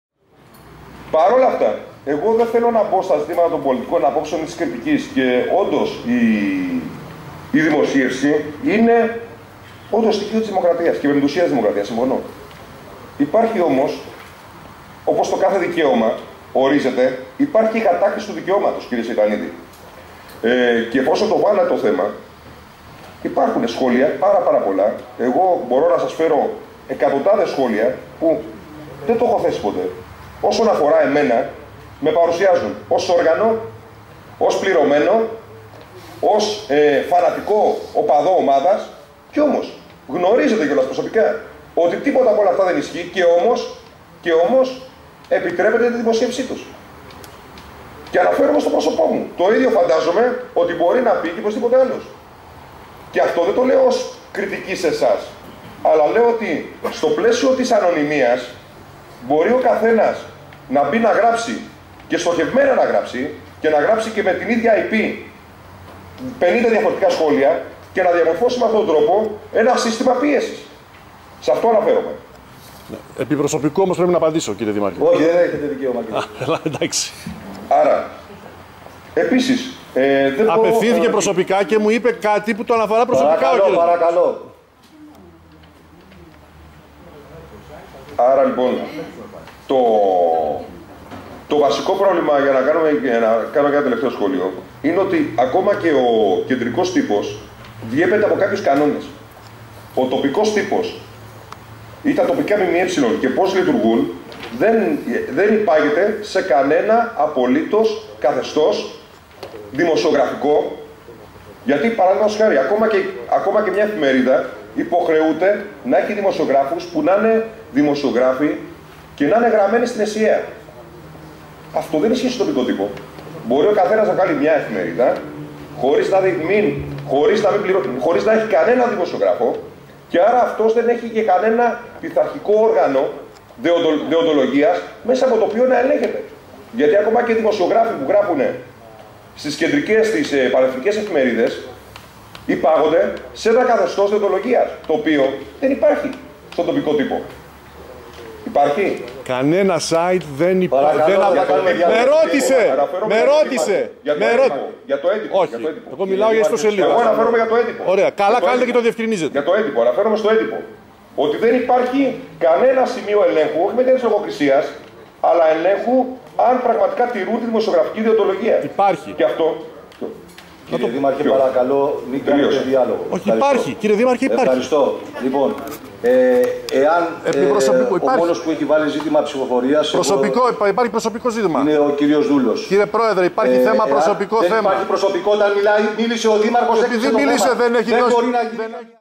Τι είπε ο Δήμαρχος για ανώνυμα σχόλια κι έλεγχο δημοσιογράφων
Δήμαρχος ανώνυμα σχόλια